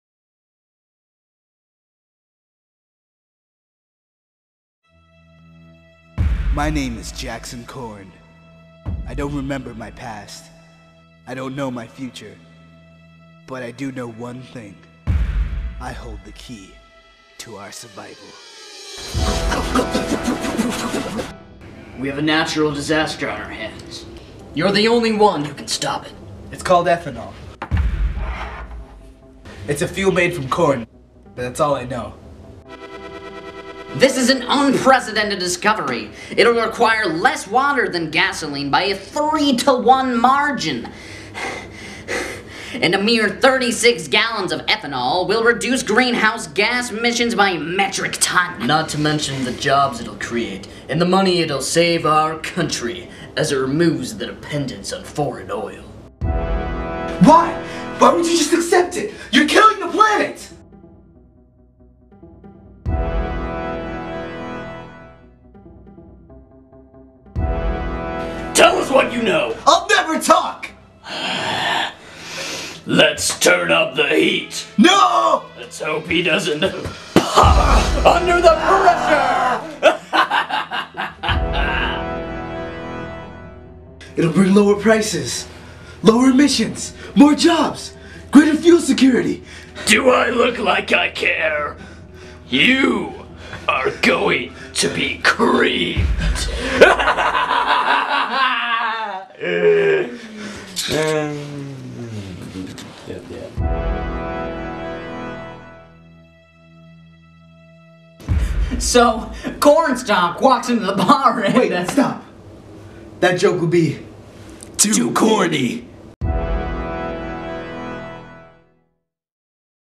The Corn Identity
Created for the Field to Fuel Video Contest in order to raise awareness about renewable fuel sources such as ethanol.

Jackson Corn awakes, alone, confused, and happens to be holding the secret to our survival.

Director of Photography - Thomas Johnson
Composer - Thomas Johnson

Special thanks to all Concordia Live members as well as Randy Holmes for putting up with us.